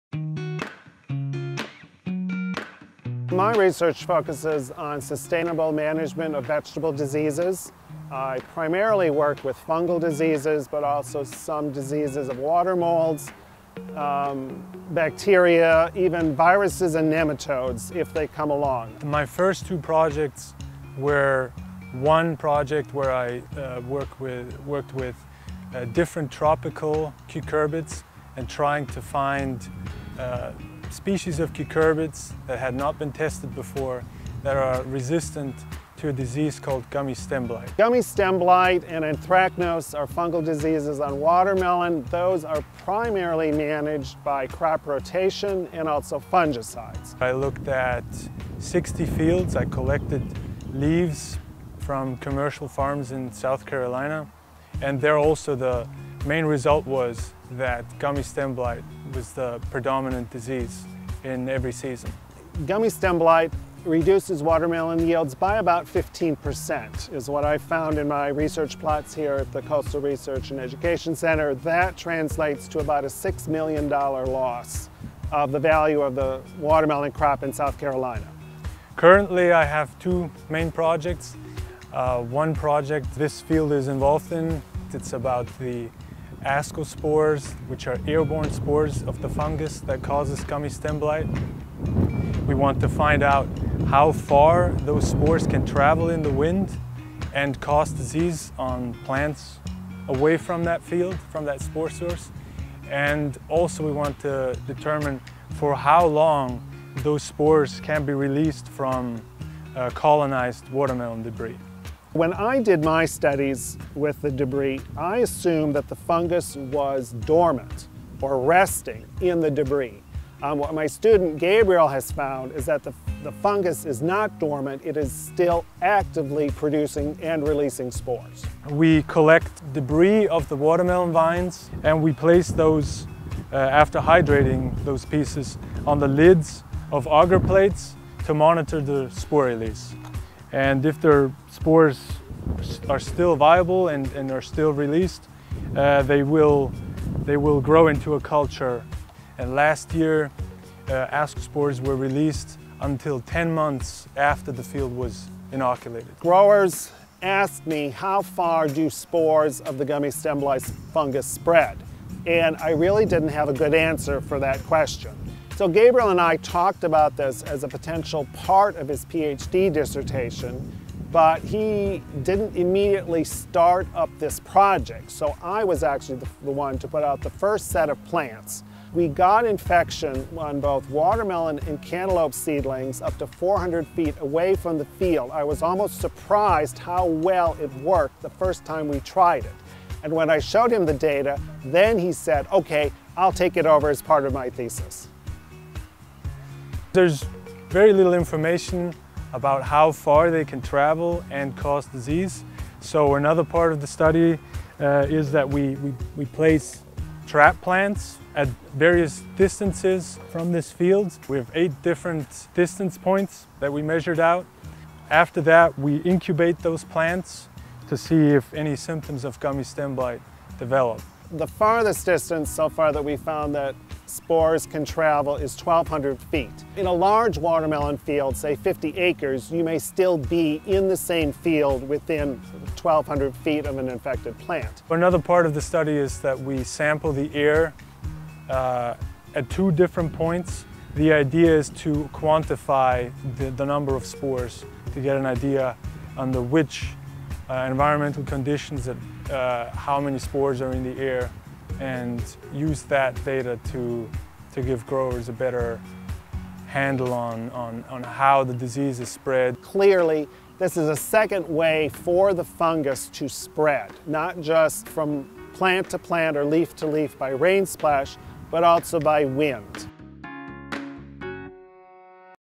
Clemson Coastal REC Gummy Stem Blight Research
Dr. Anthony Keinath and his graduate student Gabriel Rennberger are studying gummy stem, especially the ways in which it travels, in order to teach farmers best management practices to prevent the disease.

Transcript:
-My research focuses on sustainable management of vegetable diseases. I primarily work with fungal diseases, but also some diseases of water molds, also some diseases of water molds, they come along.
--My first two projects were one project where I worked with different tropical cucurbits and tried. to find species of cucurbits that had not been tested before that are resistant to a disease called gummy stem blight.
-Gummy stem blight and anthracnose are fungal diseases on watermelon.  Those are primarily managed by crop rotation and also fungicides.
--I looked at sixty fields, I collected leaves from commercial farms in South Carolina and they're also the main result was that gummy stem blight was the predominant disease in every season.
-Gummy stem blight reduces watermelon yields by about fifteen percent is what I found in my research plots here at the coastal research and education center. That translates to about a six million dollar loss of the value of the watermelon crop in South Carolina.
--Currently I have two main projects. One project this field is involved in. It's about the ascospores, which are airborne spores of the fungus that causes gummy stem blight. We want to find out how far those spores can travel in the wind and cause disease on plants away from that field, from that spore source. And also we want to determine for how long those spores can be released from colonized watermelon debris.
-When I did my studies with the debris, I assumed that the fungus was dormant, or resting, in the debris. What my student Gabriel has found is that the fungus is not dormant. It is still actively producing and releasing spores.
--We collect debris of the watermelon vines and we place those, after hydrating those pieces, on the lids of auger plates to monitor the spore release. And if their spores are still viable and are still released, they will grow into a culture. And last year, ascospores were released until 10 months after the field was inoculated.
-Growers asked me, “How far do spores of the gummy symbolize fungus spread?" and I really didn't have a good answer for that question. So, Gabriel and I talked about this as a potential part of his PhD dissertation, but he didn’t immediately start up this project, so I was actually the one to put out the first set of plants. We got infection on both watermelon and cantaloupe seedlings up to 400 feet away from the field. I was almost surprised how well it worked the first time we tried it. And when I showed him the data, then he said, “Okay, I'll take it over as part of my thesis.”
--There's very little information about how far they can travel and cause disease, so another part of the study is that we we place trap plants at various distances from this fields. We have eight different distance points that we measured out. After that, we incubate those plants to see if any symptoms of gummy stem blight develop.
-The farthest distance so far that we found that spores can travel is 1,200 feet. In a large watermelon field, say 50 acres, we may still be in the same field within 1,200 feet of an infected plant.
--But another part of the study is that we sample the air at two different points. The idea is to quantify the number of spores to get an idea under which environmental conditions, how many spores are in the air and use that data to give growers a better handle on how the disease is spread.
-Clearly this is a second way for the fungus to spread, not just from plant to plant or leaf to leaf by rain splash but also by wind.